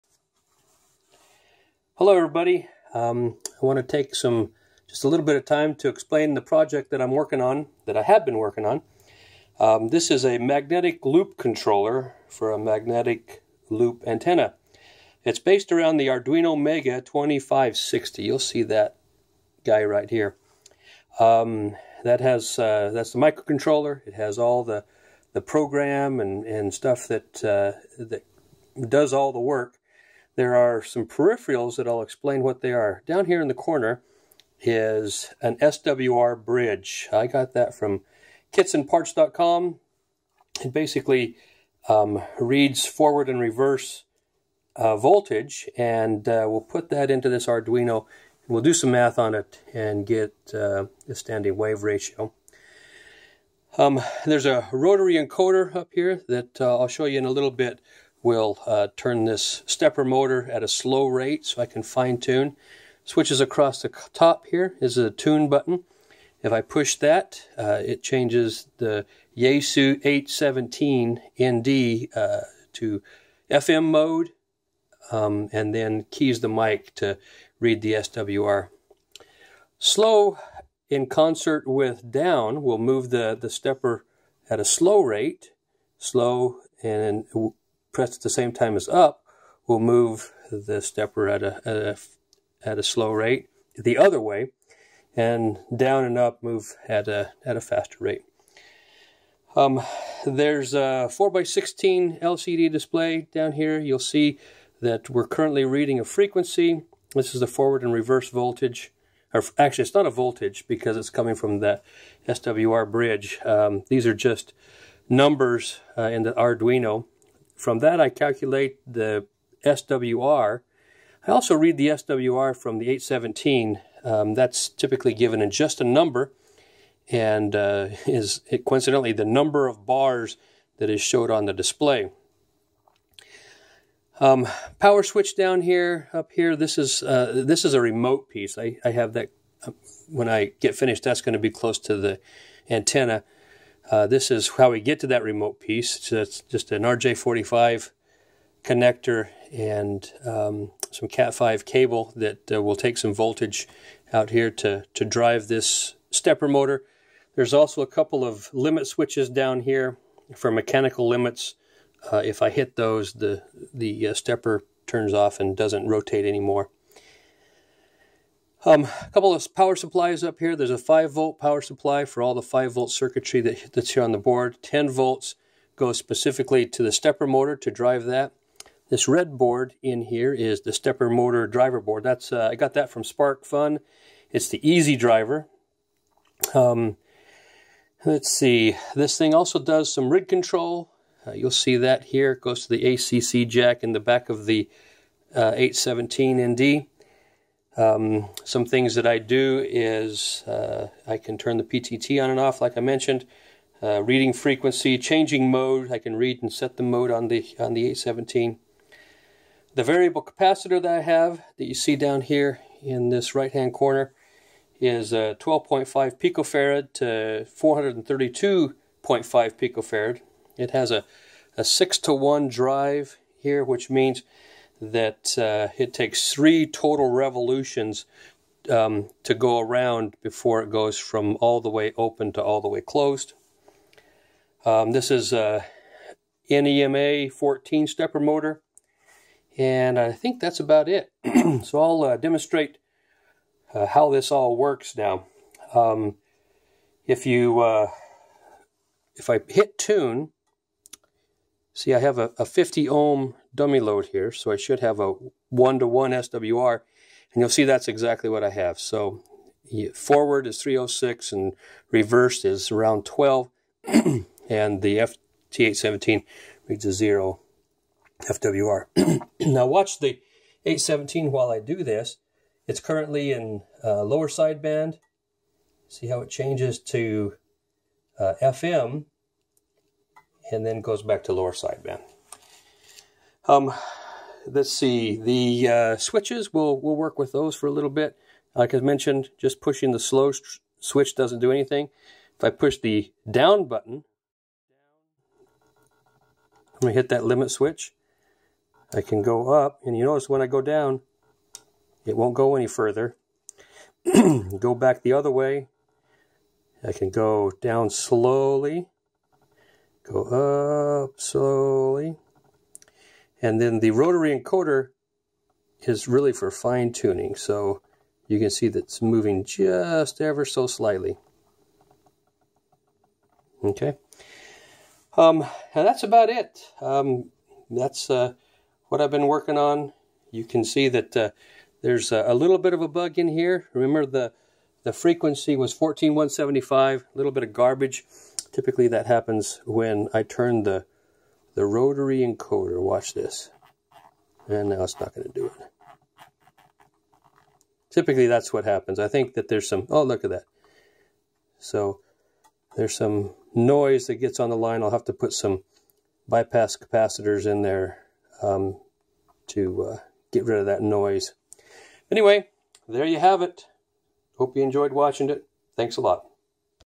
Arduino Magnetic Loop Controller Project (in progress)
This is a project that I've been working on for a while now. The guts of it is an Arduino Mega 2560. I connect this through a serial connection to the ACC jack of my Yaesu FT-817nd radio. The Arduino can read the frequency from the 817 and display it on a 4x16 LCD along with other information. When the [Tune] button is pushed, the mode that the 817 is currently in is stored in memory and then the mode is switched to FM and the PTT is keyed. The forward and reverse voltages are input to the Arduino through the analog to digital pins and the values are converted to a number. The forward and reverse numbers result in an standing wave ratio (SWR) that is displayed on the LCD.

Besides the [Tune] button, there are three other momentary contact switches. One is the [Up] button which turns the stepper motor counter clockwise, the [Down] button that turns the stepper motor clockwise, and a [Slow] button that when pushed with one of the other two, slows the motion of the stepper for finer tuning.

There is also a rotary encoder that, when turned right or left turns the stepper in the proper direction at a very slow rate.

The stepper driver board is a Sparkfun Easydriver and is fed with a 10v DC-DC converter. The main power to the unit is 13.5v. There is also a 5v DC-DC converter that feeds the Arduino and powers all the logic devices on the board.

The stepper is a NEMA-14 motor and it is attached to a 12.5pf - 432.5pf air-variable capacitor that has a 6:1 gear ratio. This means that the stepper will need to turn three full turns for the capacitor to go from a fully open position to a fully closed position. On each mechanical limit of the capacitor there are limit switches to turn off the stepper when that physical limit is reached.

There is a lot more work to do, but I'm about 1/3 of the way done so I thought I'd make a video.

I hope you liked it. I'll update my progress with more videos in the future.